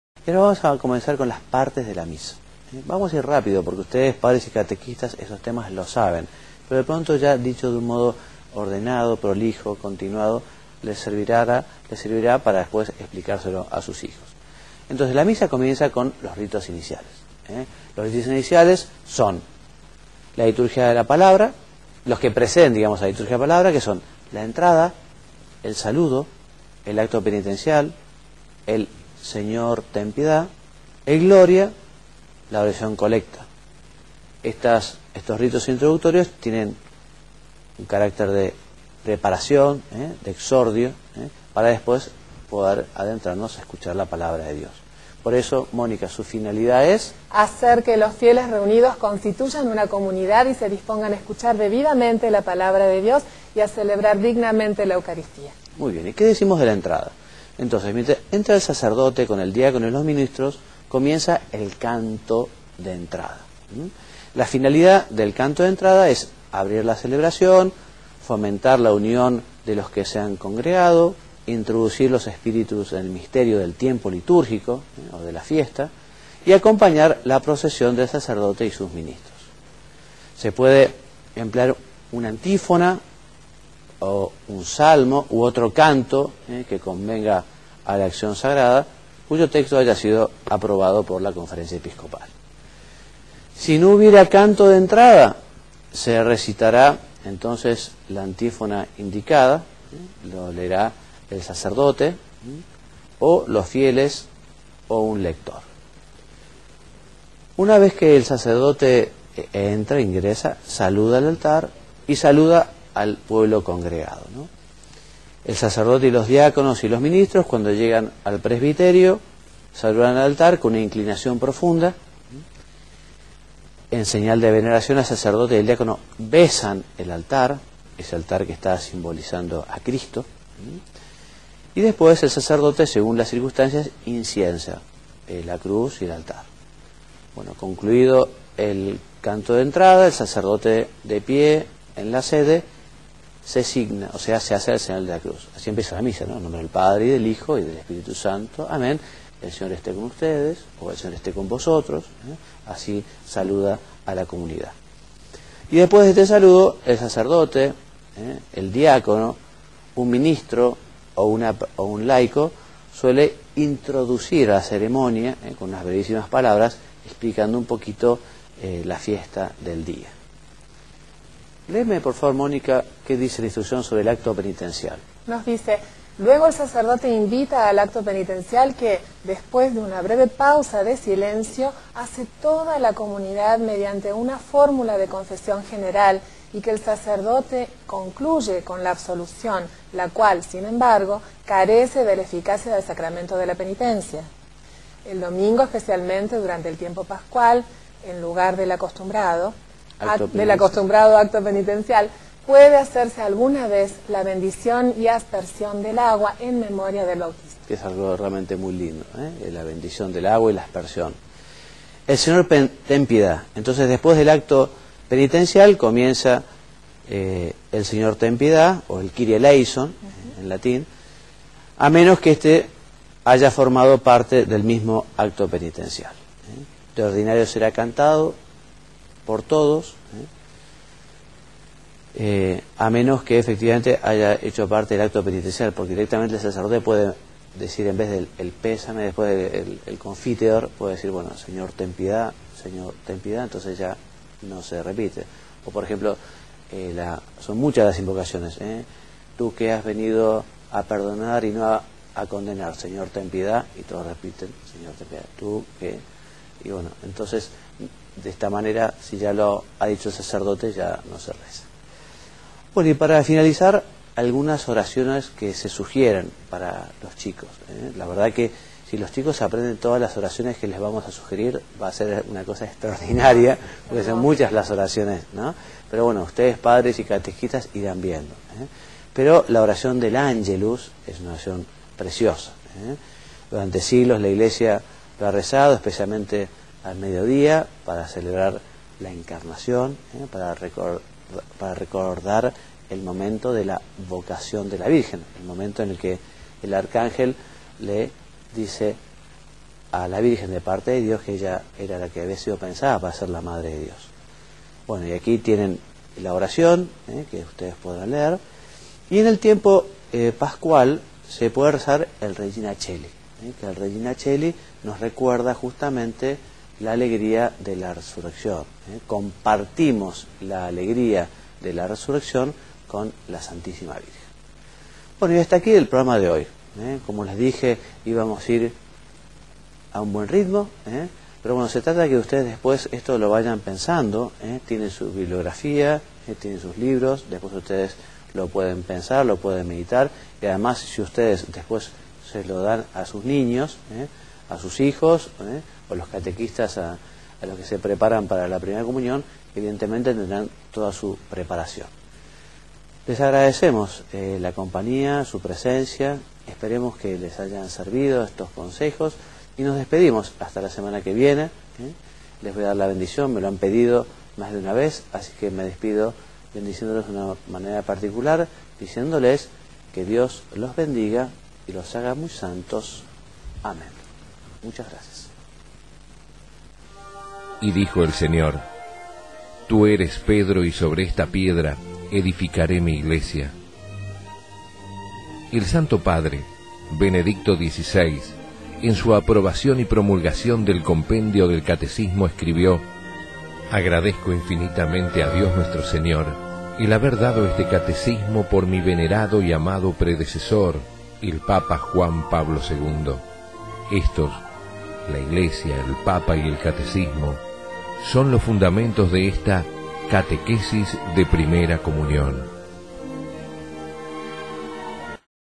Cat 05 c IGMR 46 a 52
Curso para padres y catequistas de primera comunión en 40 programas. Se compone sobre tres grandes temas: la Historia Sagrada, las verdades del Compendio y sobre la instrucción general para el Misal Romano. Quinto tema Instrucción general del Misal Romano número 46 a 52. La Misa comienza con los ritos iniciales.